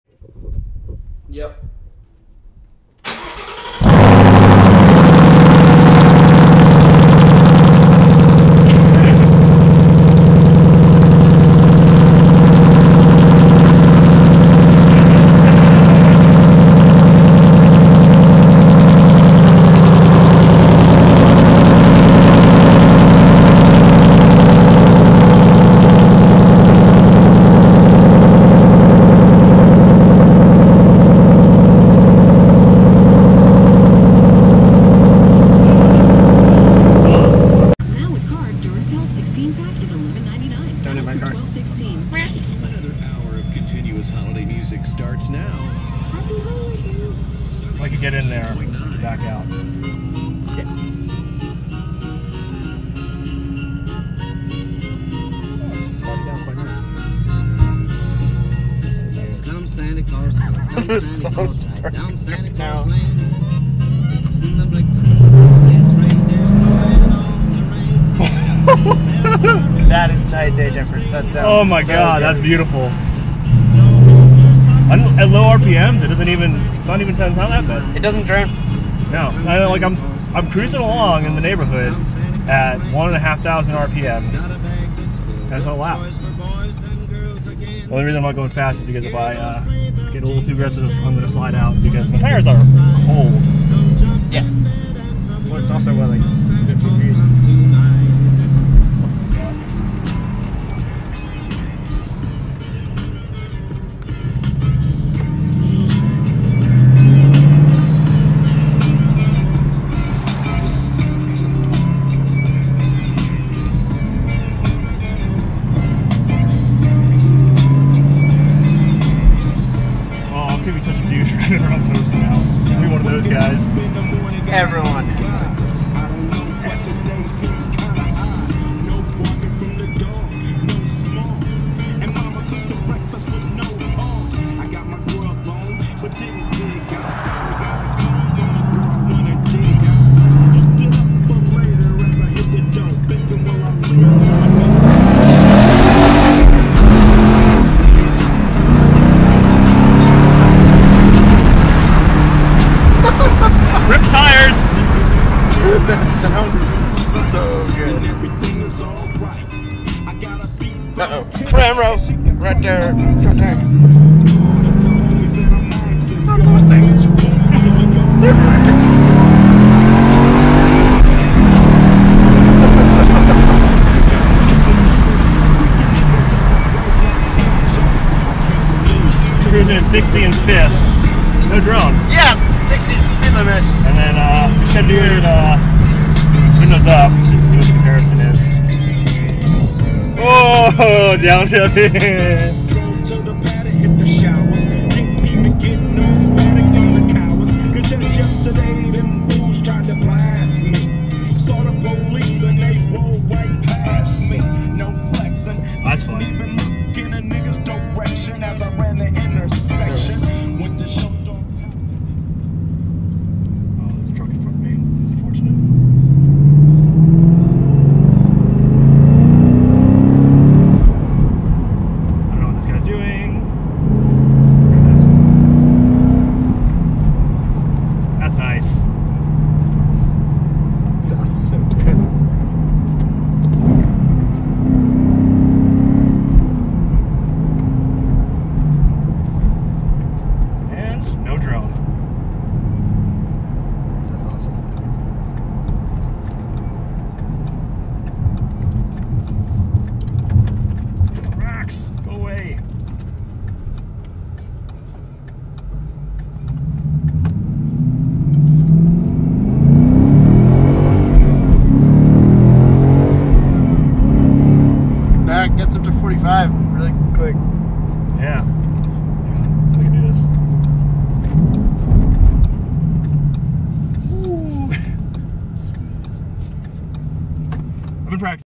2017 Mustang GT Ford Performance Exhaust (Cold Start & First Drive)
My buddy Brian installed his Ford Performance Exhaust on his 2017 S550 Mustang GT. The exhaust is essentially a Borla S-Type exahust but the x-pipe does not have a resonator. Sounds fantastic! It sort of was more than my microphone could handle in fact. Easy install, as it took only about an hour and a half to take the OEM exhaust off and put this on.